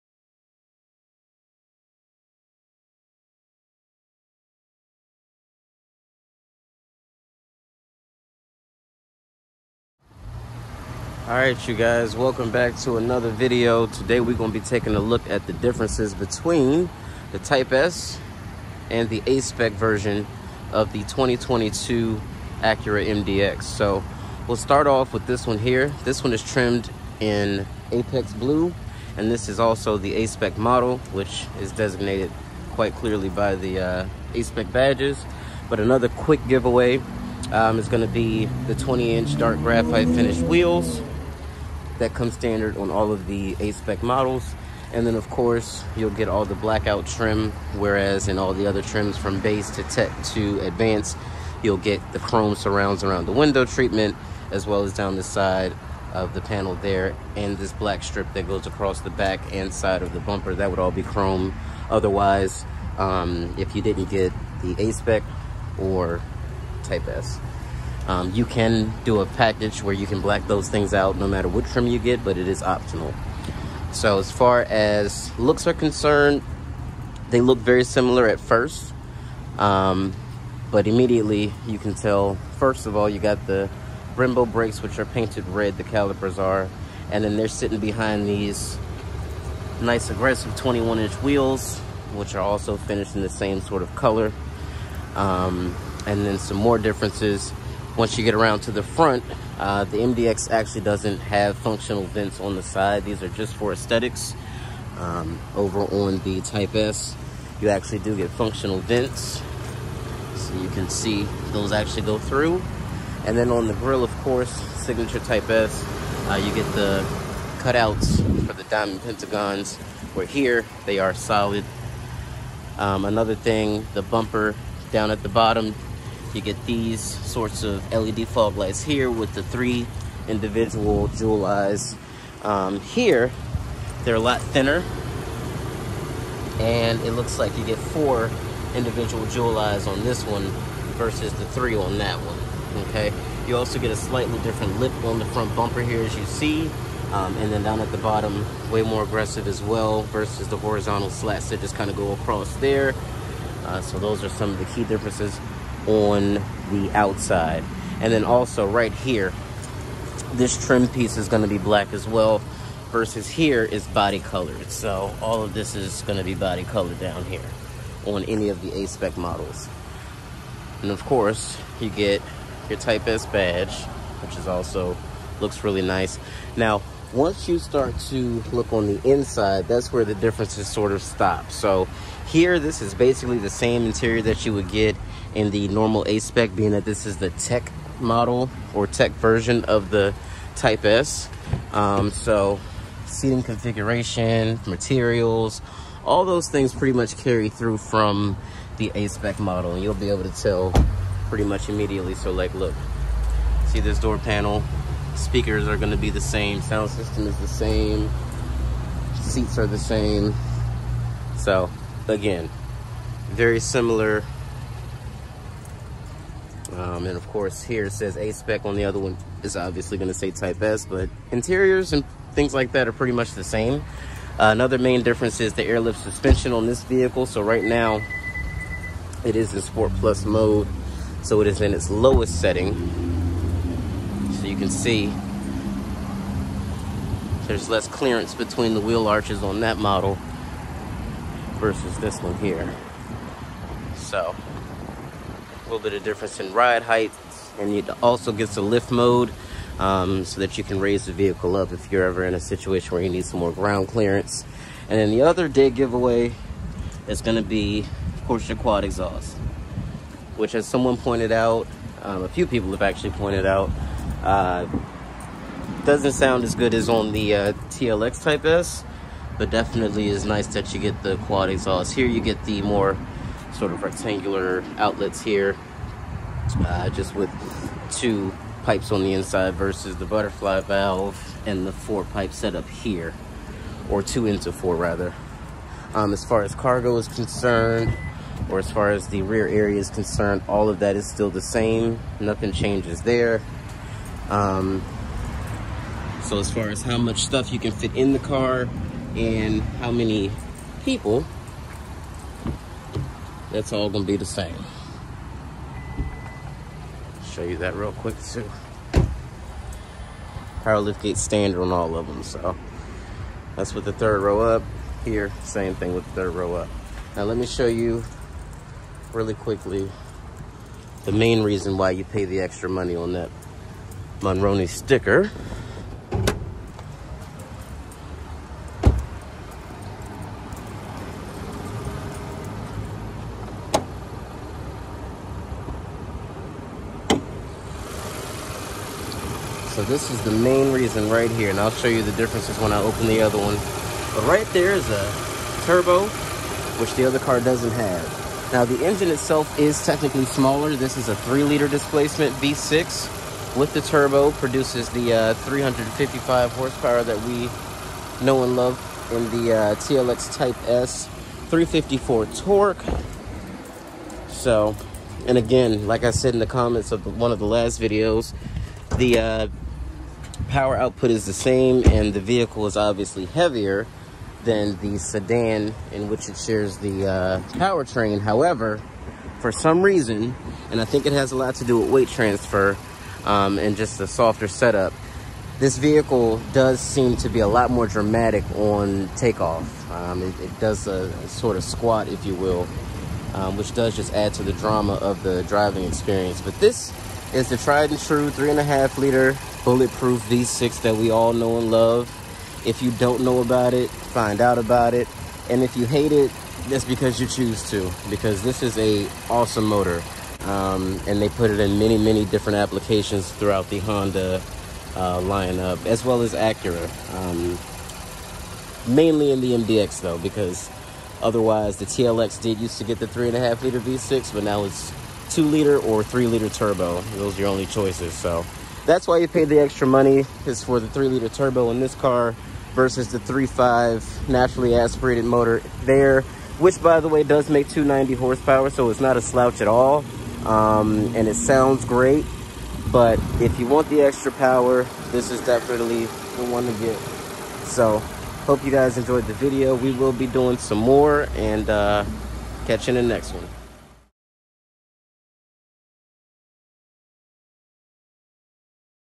2022 MDX Comparison | A-Spec 🥶 vs. Type-S 🔥
In this video we do a quick comparison of the 2022 MDX A-Spec & Type-S. We go over what makes them very different but so similar at the same time. Hope you enjoy the video!